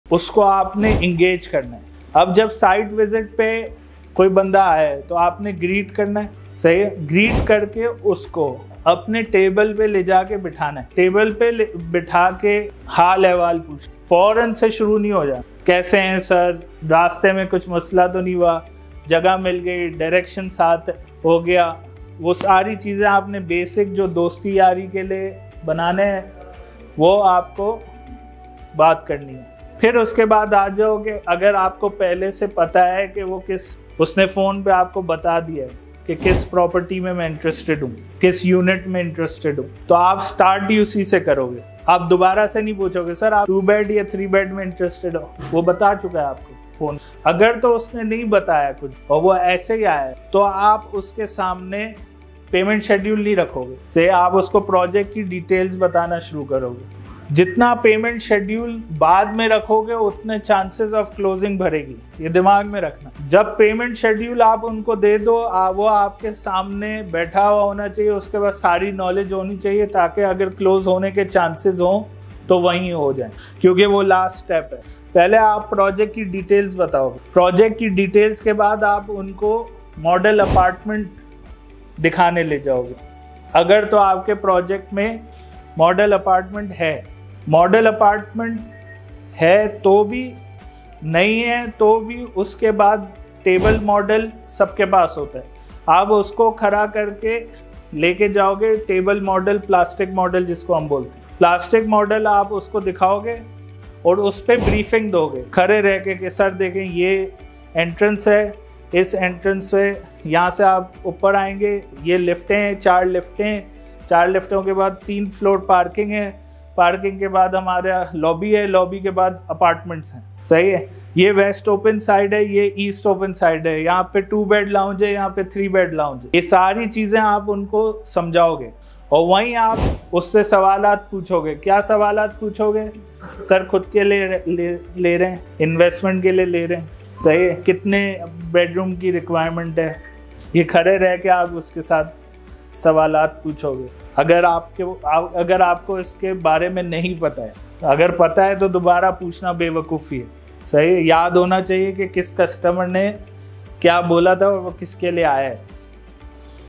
How to brief Client for Under Construction Real Estate Project | Site Visit. Step by Step process
When clients visit your site, you must greet them all warmly and then build rapport by making small talk and asking how they are doing. To avoid appearing unprepared and unprofessional, do not ask your client the same questions again if you spoke with them over the phone prior to your meeting if they had told you what they are looking for in an apartment. The second step is to show them the project details, the table model apartment, the apartment features, and the complex facilities. The third step is to ask the crucial questions: are you purchasing this for yourself? Do you intend to invest in this property? Finally, last but not least, always remember your customers' names, if you had spoken to them over the phone before the in-person meeting, and what your client’s requirements are.